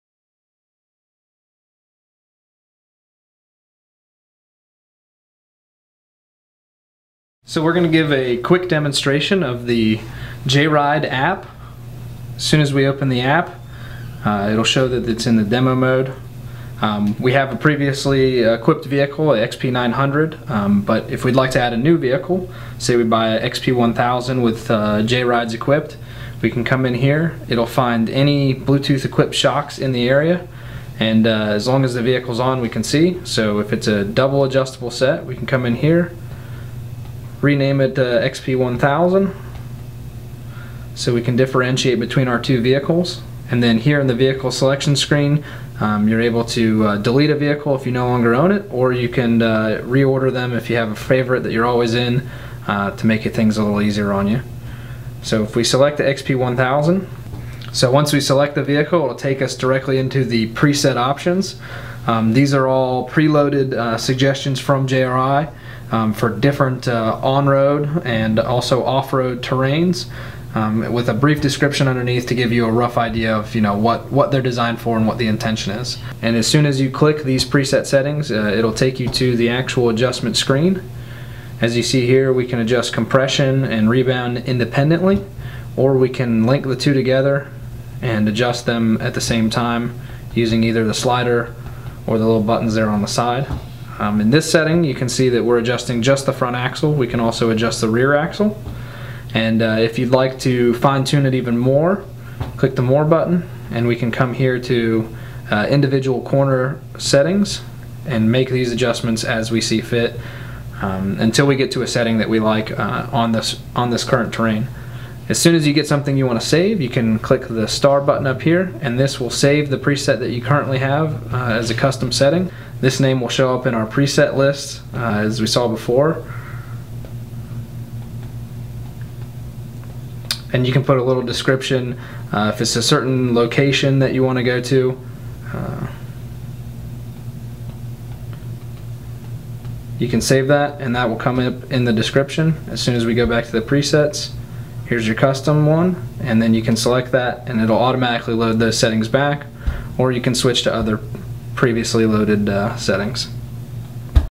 JRi Shocks Smart phone app tutorial
JRi Shocks introduces the first ever smart phone adjustable shock system for a UTV.  Here JRi Demonstrates the features and functionality of the JRide system